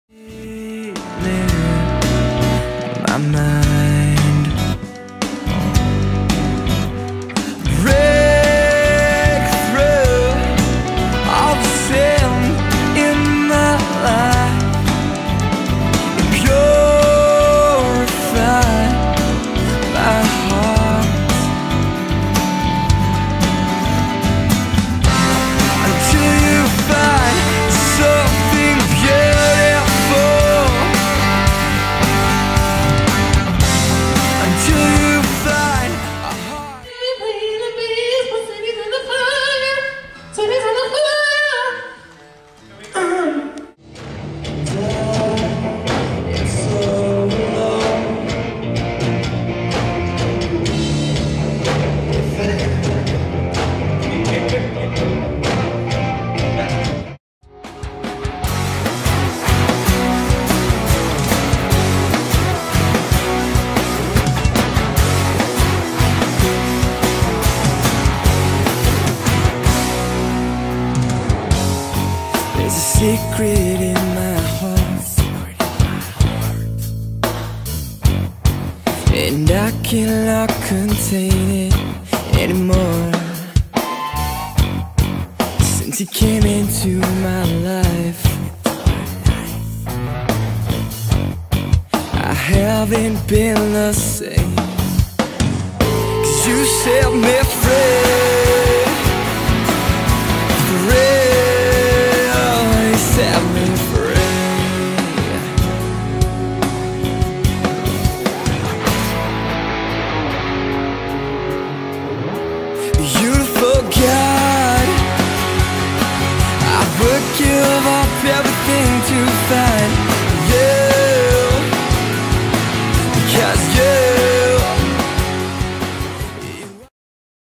On tour part #2.m4v
On tour with Cities Under Fire and Define The Line